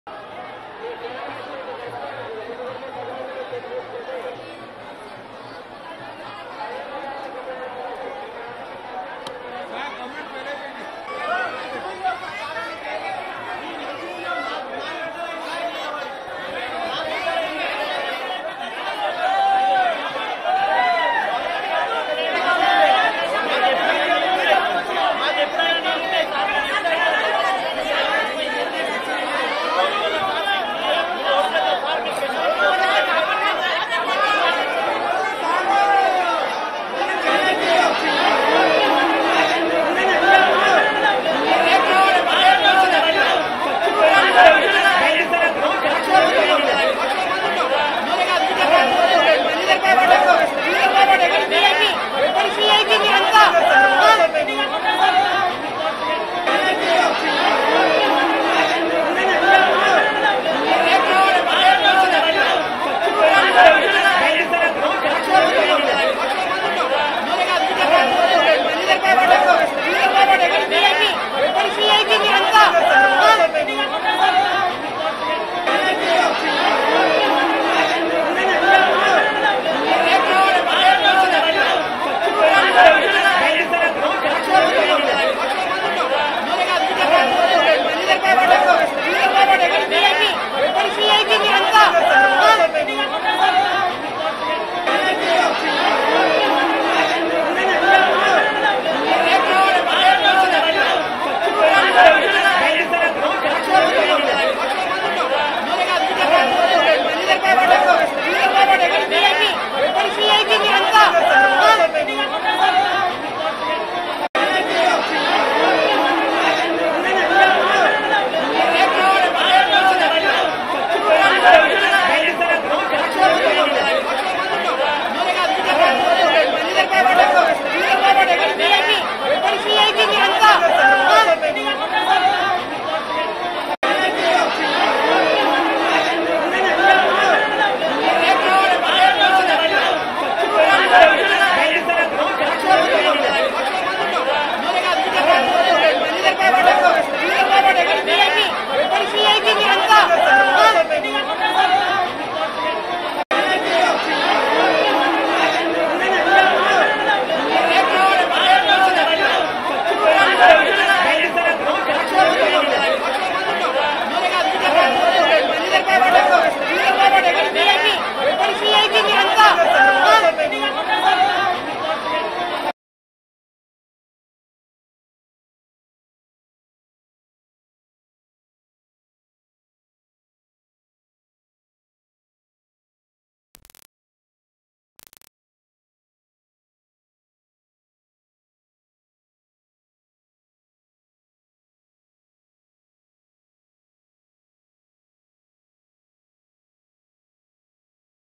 Medaram Jatara 2024 | Public Fires On Ex Minister Errabelli Dayakar Rao | V6 News
బలగంతో వచ్చి..